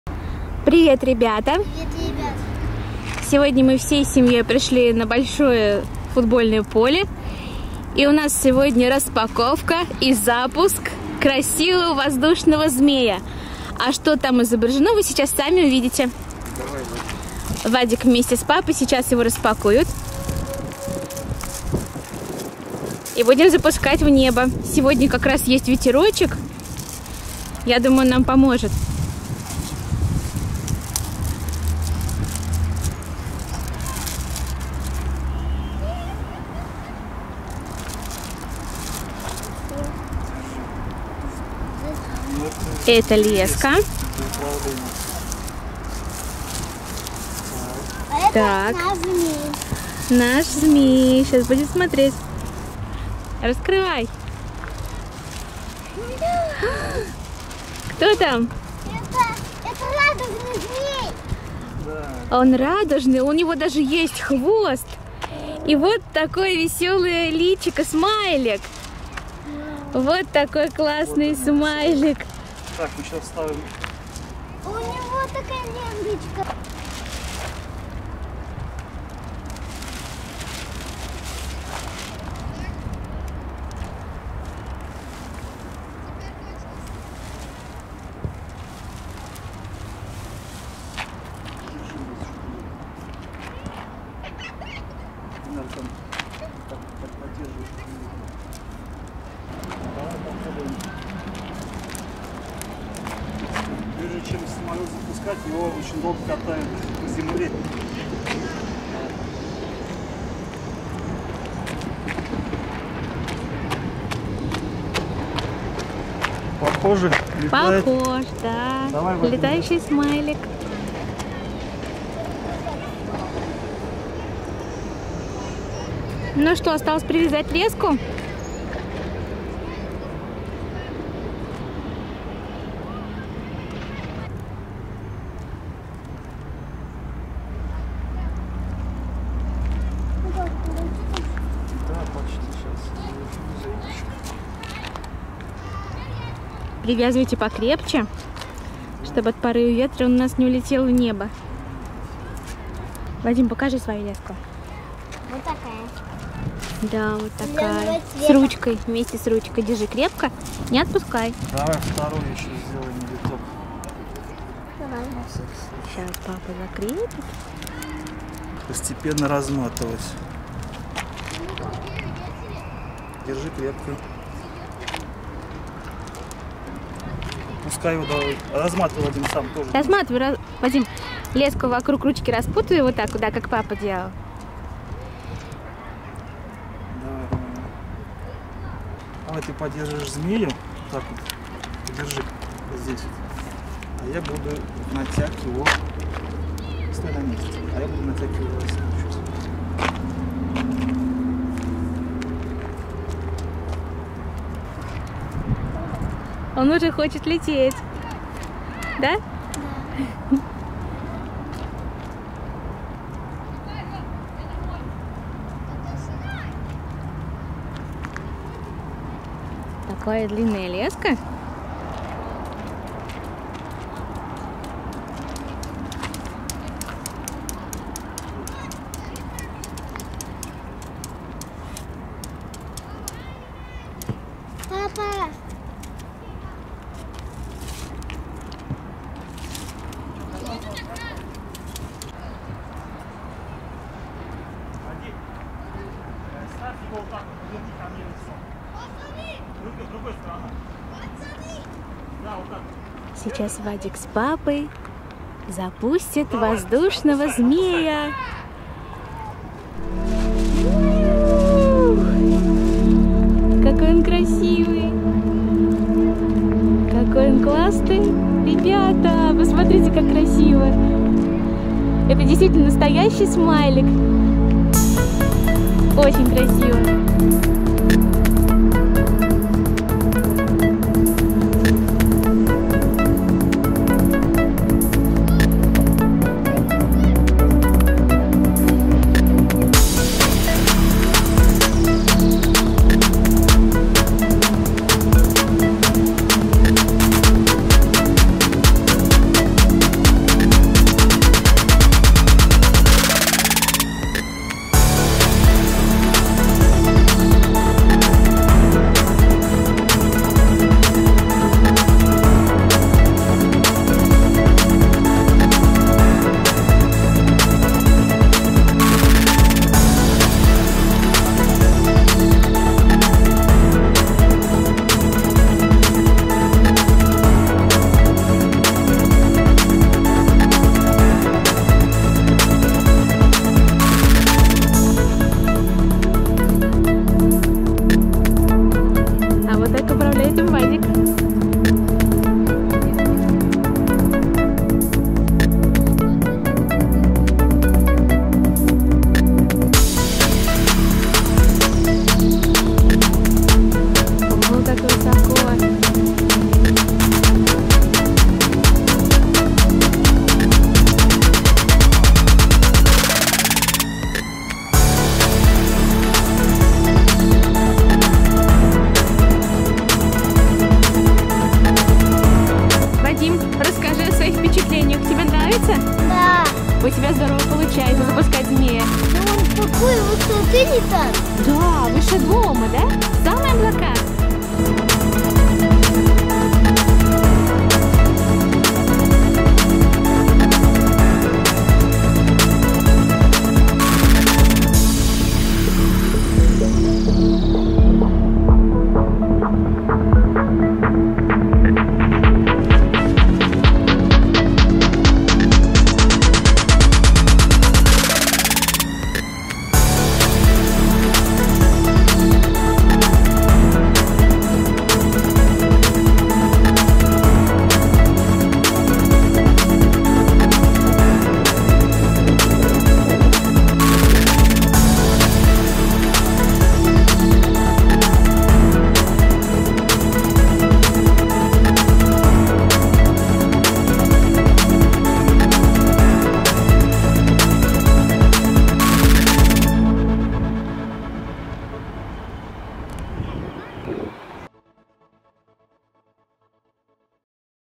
Мы с Вадиком распаковываем и запускаем воздушного змея
Ребята , приглашаем посмотреть полное видео с запуском воздушного змея, это клёво !